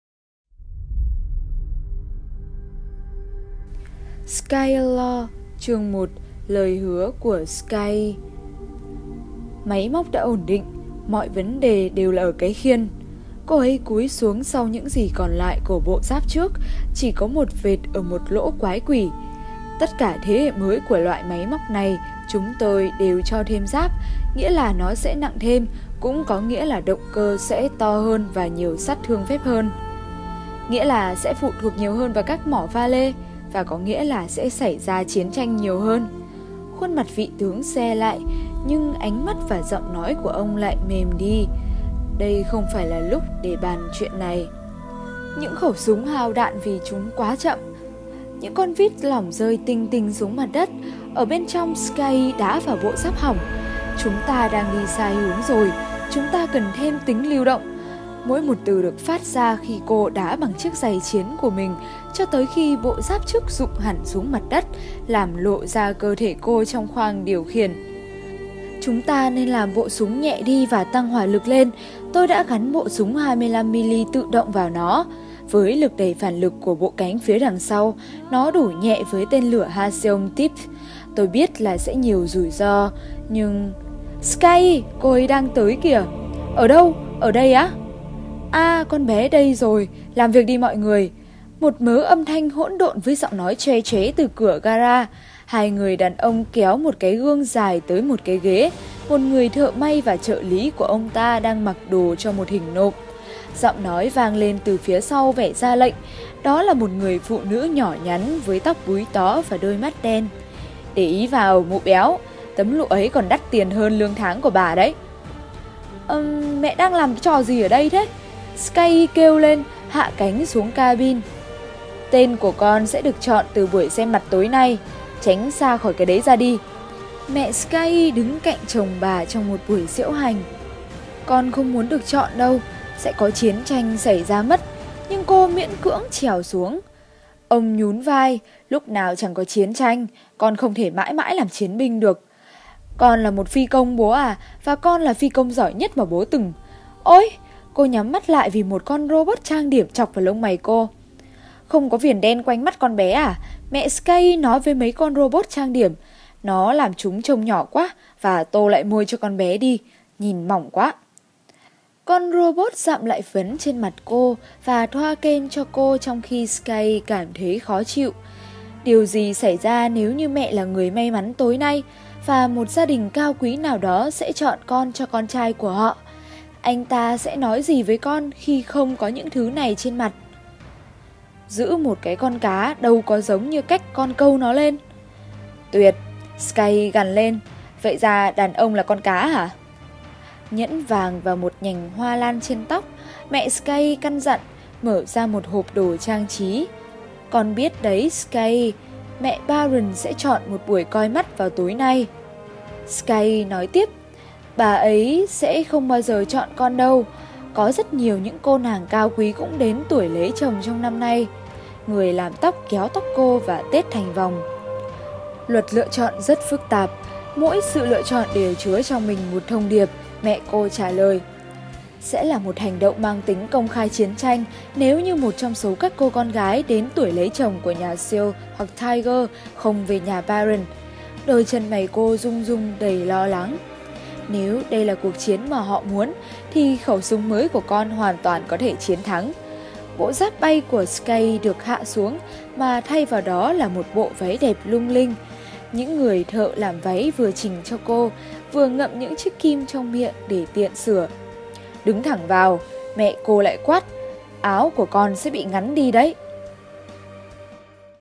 [Truyện Vainglory] Skye lore 11: Lời hứa của Skye
[Truyện Vainglory] Skye lore: Lời hứa của Skye
Con robot dặm lại phấn trên mặt cô và thoa kem cho cô trong kho Skye cảm thấy khó chịu. "Điều gì xảy ra nếu như mẹ là người may mắn tối nay, và một gia đình cao quý nào đó sẽ chọn con cho con trai của họ? Anh ta sẽ nói gì với con khi không có những thứ này trên mặt?"